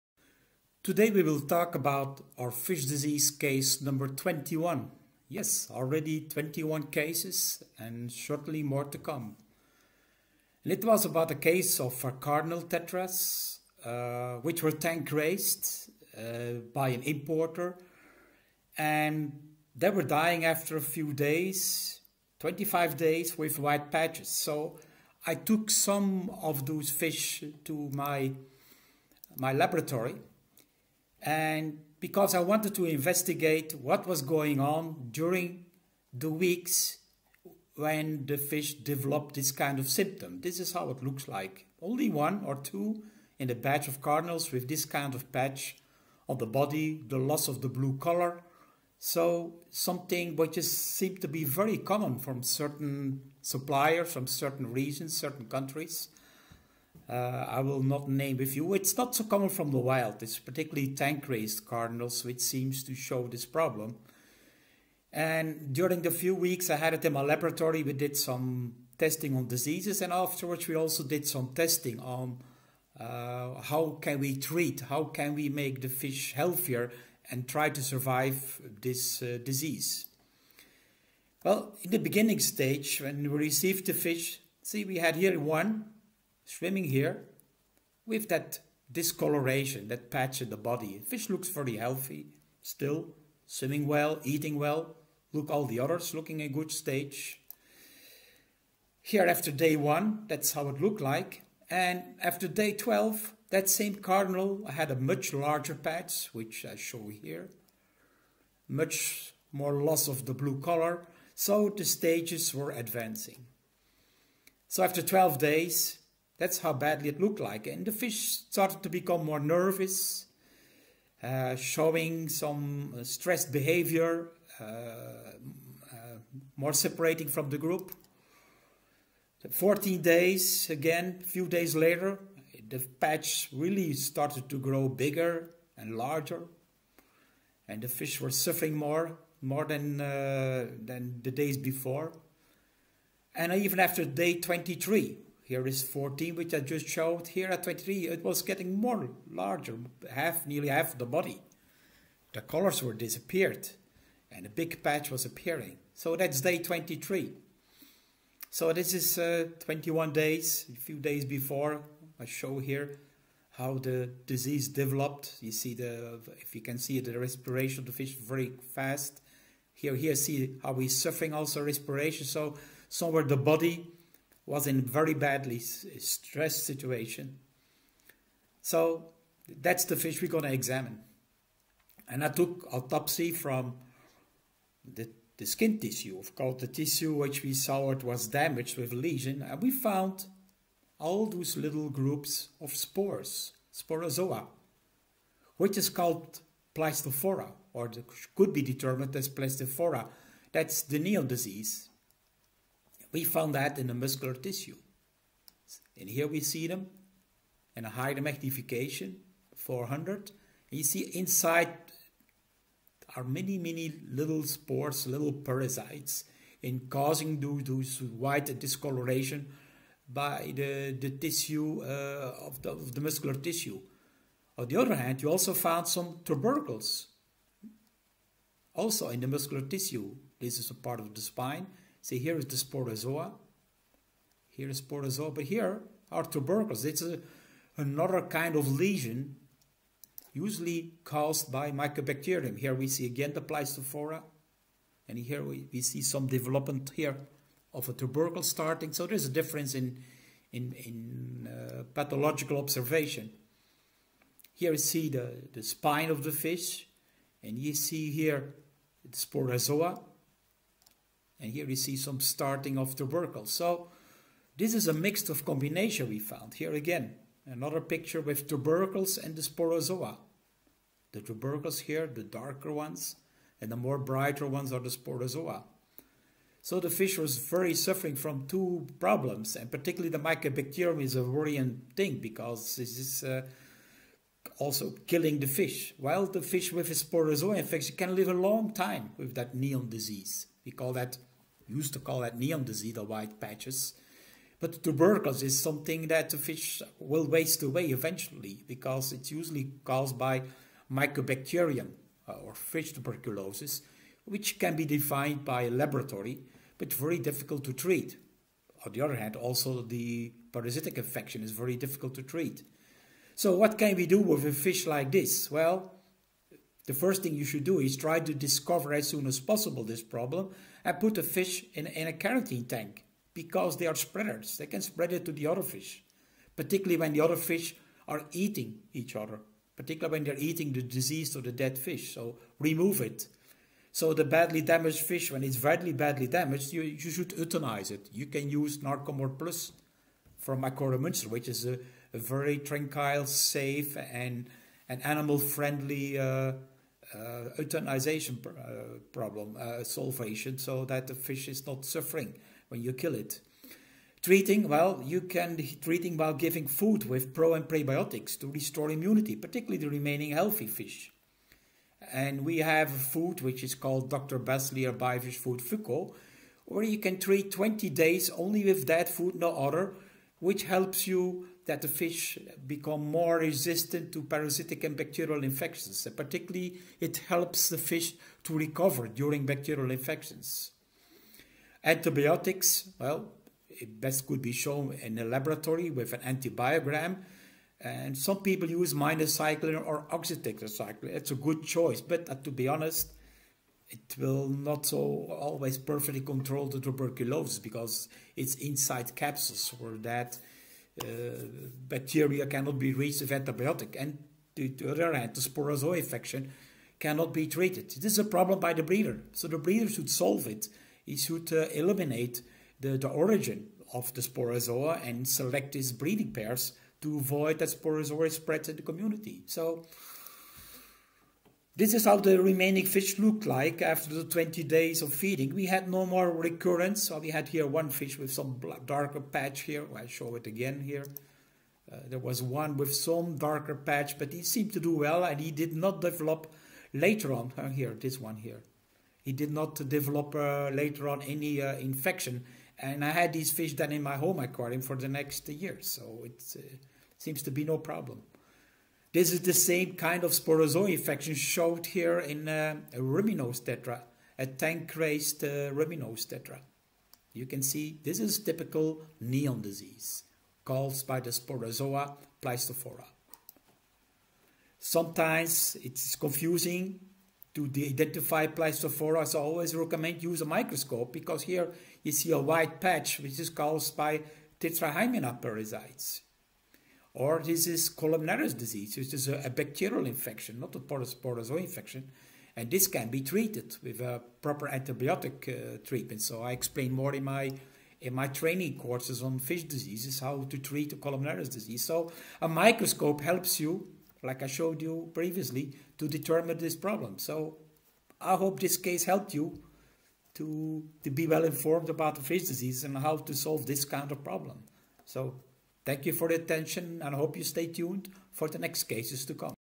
Fish disease case nr. 21: Cardinal tetra, Paracheirodon axelrodi, with bacterial infection
In my recent (2021) research on fish diseases I have found often tankraised Cardinal tetra (Paracheirodon axelrodi) with discoloration, patches (sometimes necrotic) in the skin & muscular tissues. I will show the different stages of the disease and my microscopic observations. Also advice for treatment will be presented.